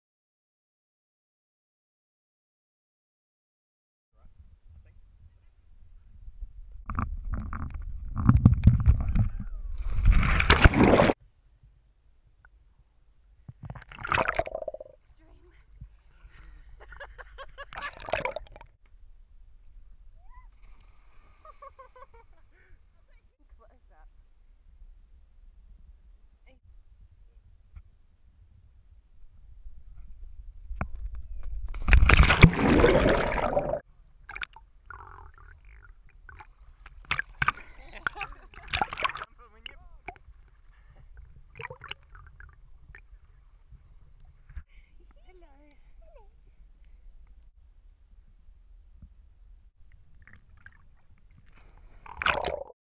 Taylorville
Swimming with friends at the popular Taylorville swimming hole near Greymouth